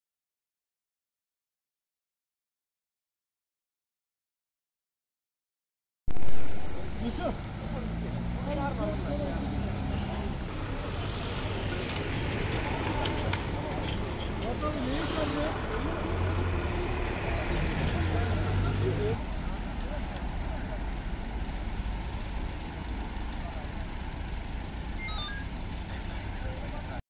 Aydın'da ölümlü kaza!
Aydın'ın Söke ilçesinde meydana gelen trafik kazasında yaralanan motosiklet sürücüsü, kaldırıldığı hastanede yaşamını yitirdi.